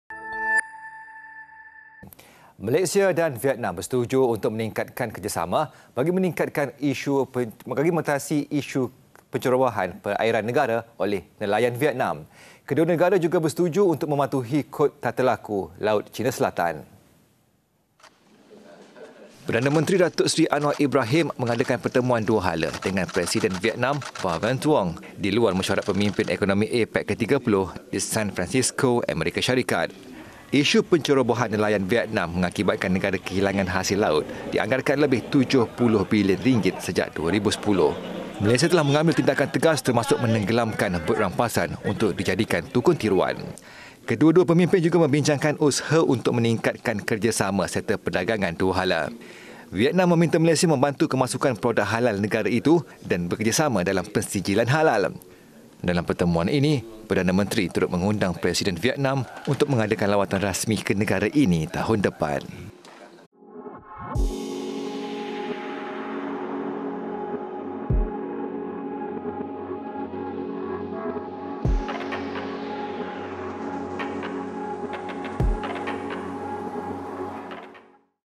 MALAYSIA-VIETNAM | Malaysia, Vietnam Setuju Tingkat Kerjasama Atasi Isu Pencerbohan Perairan Negara
Malaysia dan Vietnam bersetuju untuk meningkatkan kerjasama bagi mengatasi isu pencerobohan perairan negara oleh nelayan Vietnam.